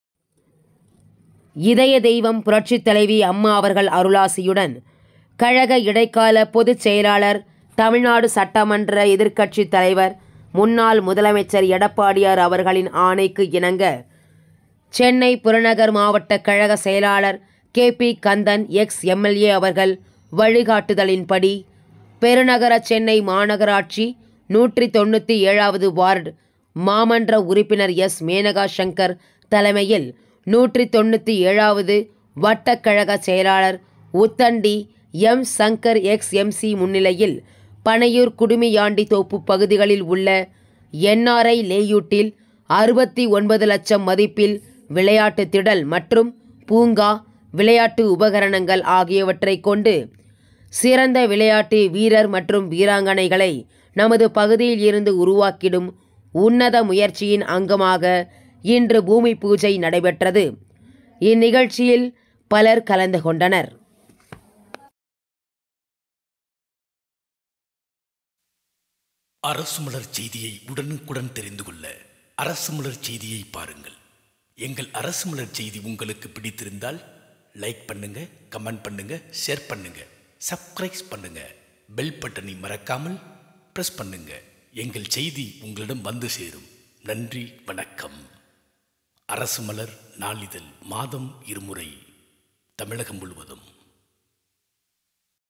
குடுமியாண்டி தோப்பு பகுதிகளில் உள்ள NRI லேயூட் இல் 69,0000 லட்சம் மதிப்பில் விளையாட்டு திடல்
இதயதெய்வம் புரட்சித்தலைவி அம்மா அவர்கள் அருளாசியுடன்
கழக இடைக்கால பொதுச்செயலாளர் தமிழ்நாடு சட்டமன்ற எதிர்கட்சி தலைவர் முன்னாள் முதலமைச்சர் எடப்பாடியார் அவர்களின் ஆணைக்கிணங்க
சென்னை புறநகர் மாவட்டக் கழக செயலாளர் 𝐊.𝐏.கந்தன் 𝐄𝐱.𝐌𝐋𝐀.., அவர்கள் வழிகாட்டுதலின்படி பெருநகர சென்னை மாநகராட்சி 197 வது வார்டு மாமன்ற உறுப்பினர் S.மேனகா சங்கர் தலைமையில் 197 வது வட்டக் கழகச் செயலாளர் உத்தண்டி M.சங்கர்ExMC முன்னிலையில் பனையூர் குடுமியாண்டி தோப்பு பகுதிகளில் உள்ள NRI லேயூட் இல் 69,0000 லட்சம் மதிப்பில் விளையாட்டு திடல் மற்றும் பூங்கா விளையாட்டு உபகரணங்கள் ஆகியவற்றை கொண்டு சிறந்த விளையாட்டு வீரர் வீராங்கனைகளை நமது பகுதியிலிருந்து உருவாக்கிடும் உன்னத முயற்சியின் அங்கமாக இன்று பூமி பூஜை நடைபெற்றது இந்த நிகழ்ச்சியில் பலர் கலந்து கொண்டனர்,